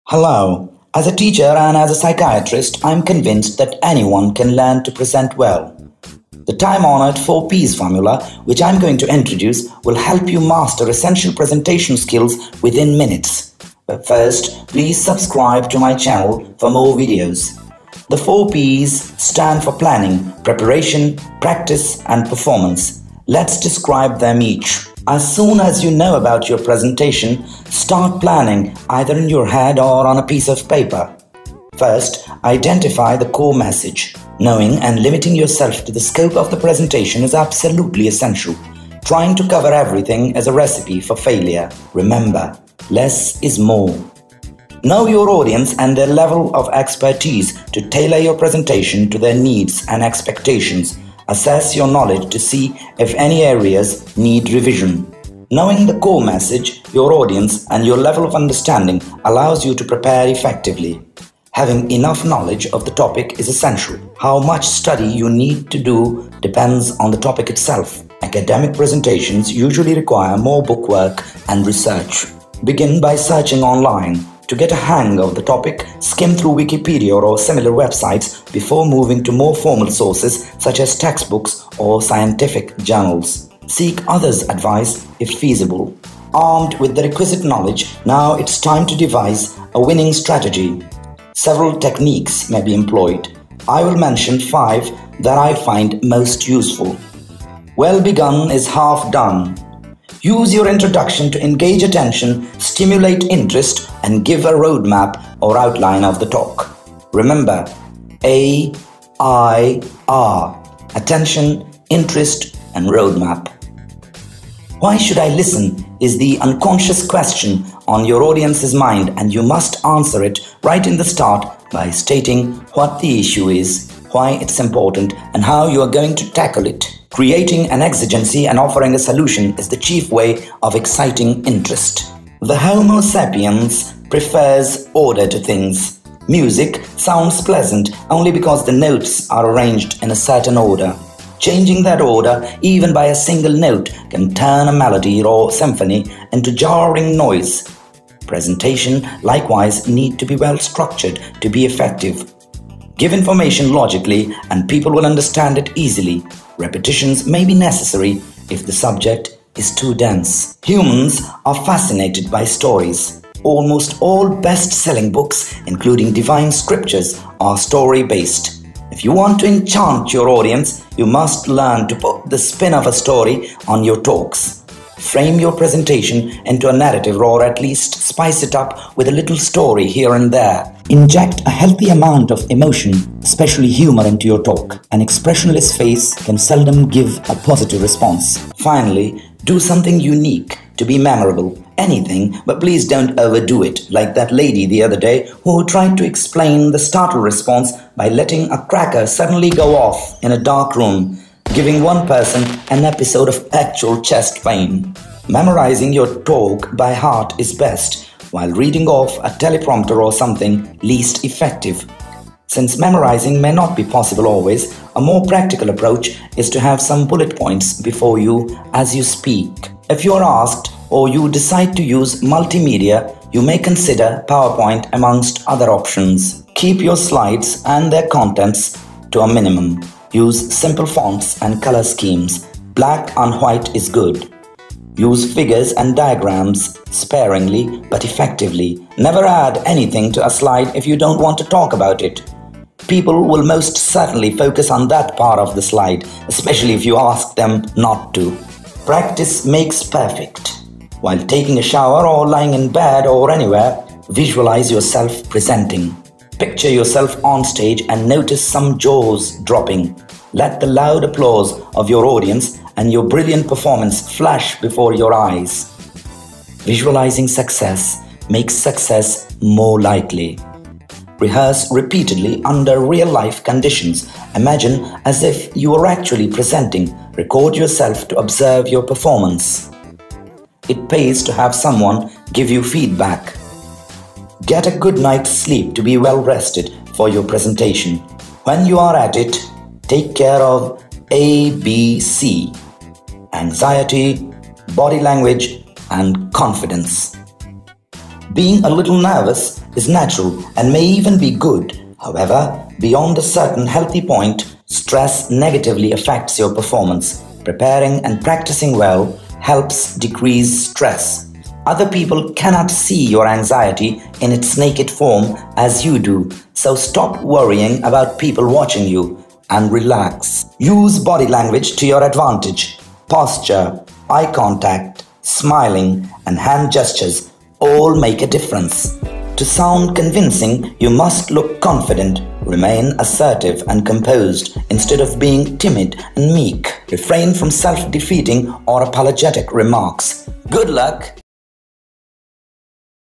How to Make a Great Presentation within Minutes
Being able to present effectively is a crucial skill for growth and success in any career. By mastering a few essential skills of presentation, you can become a great presenter.